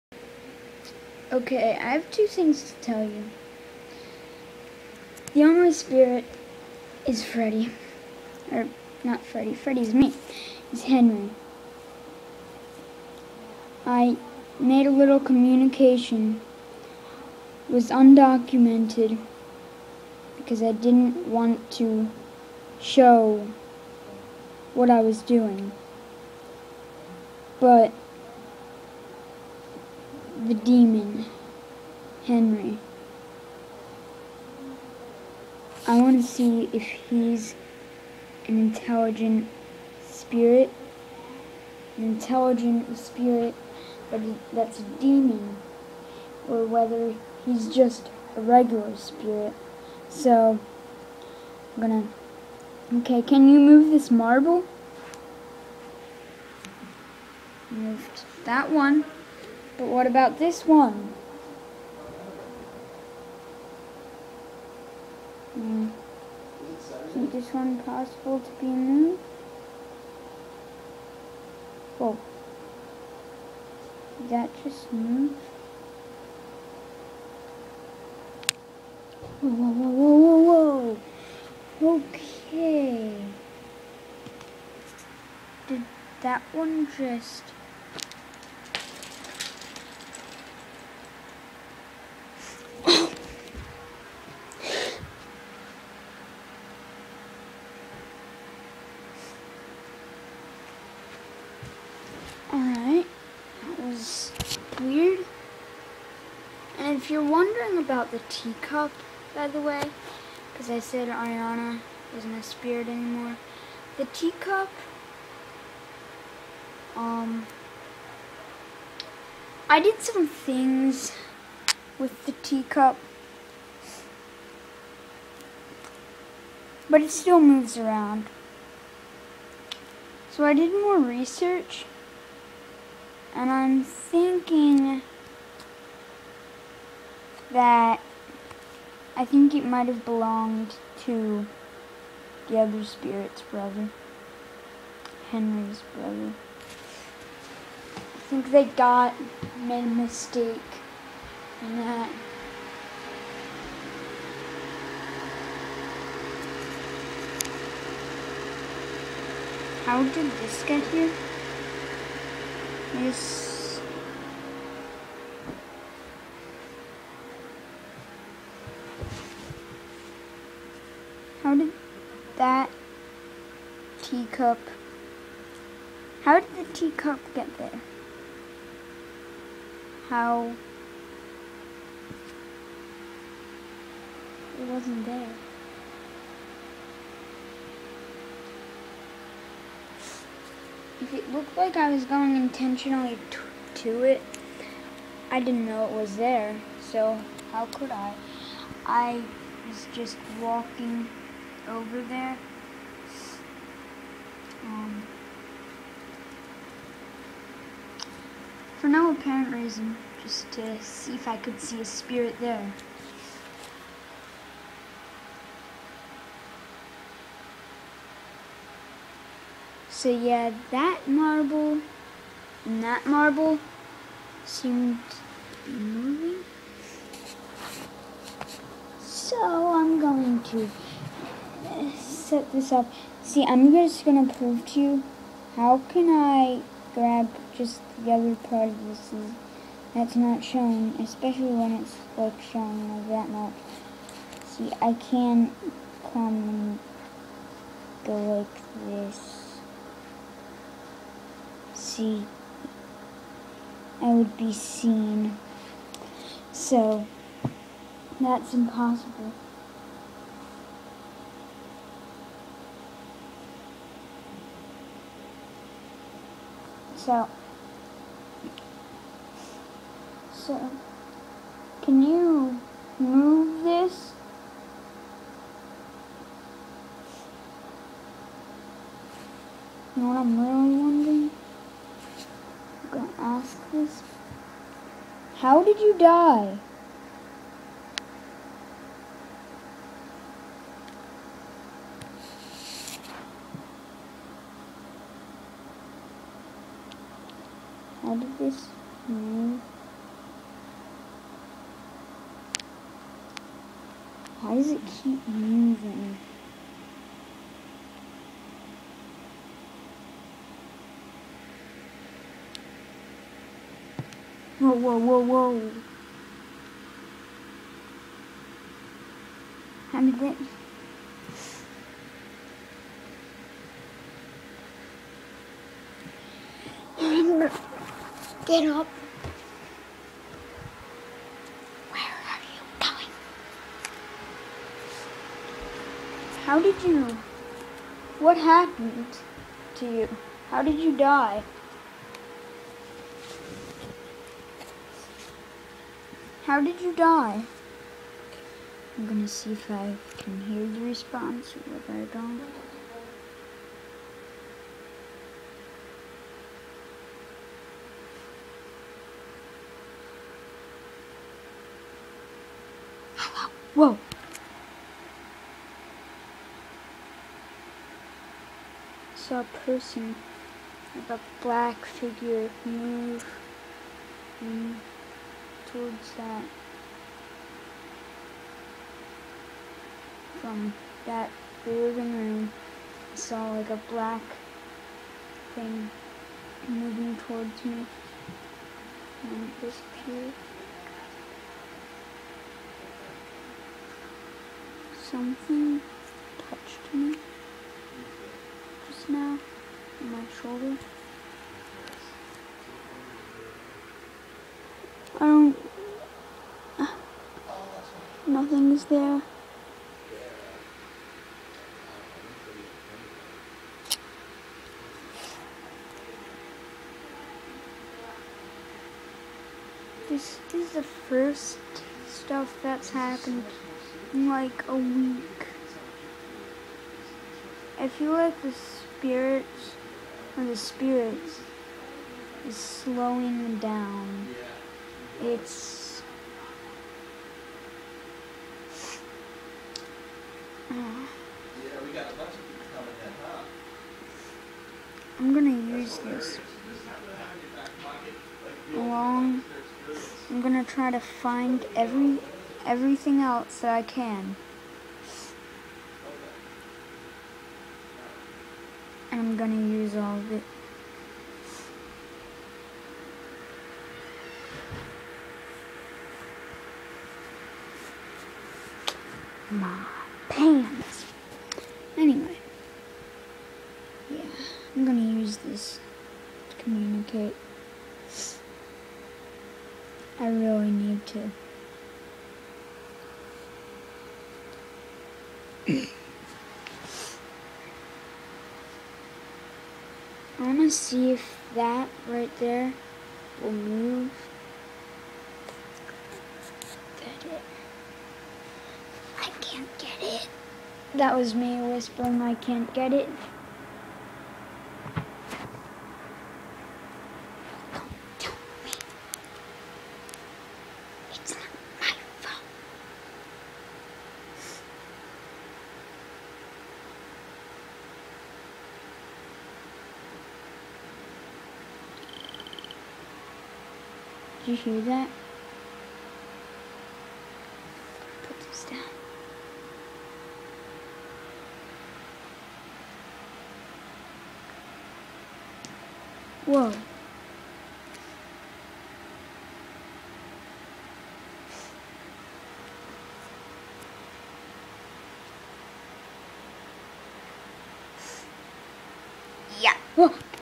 Intelligent spirit
I'm testing to see if Henry is an intelligent spirit.